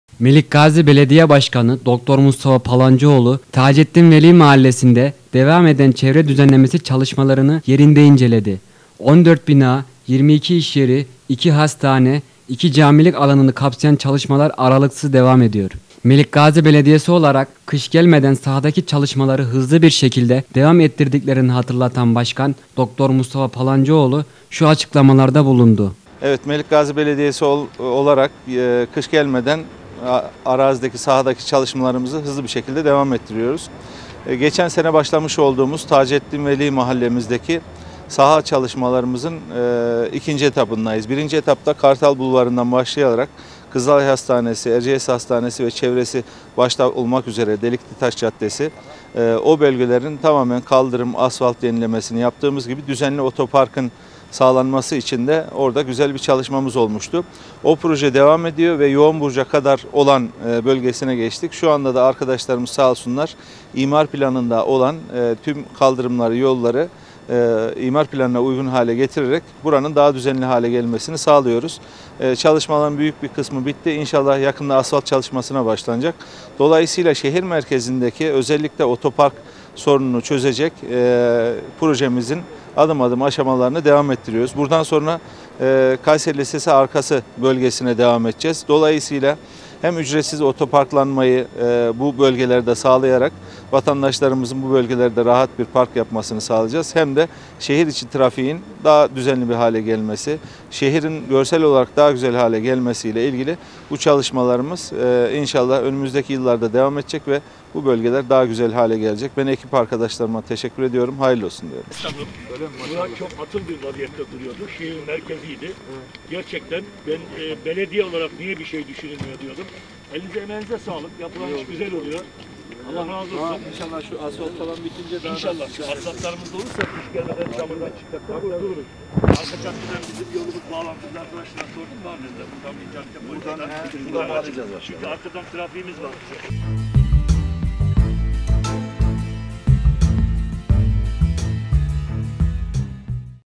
BU BAŞKAN DURMUYOR!
Melikgazi Belediye Başkanı Dr. Mustafa Palancıoğlu, Tacettin Veli Mahallesi’nde devam eden çevre düzenlemesi çalışmalarını yerinde inceledi. 14 bina, 22 işyeri, 2 hastane, 2 camilik alanı kapsayan çalışmalar aralıksız devam ediyor.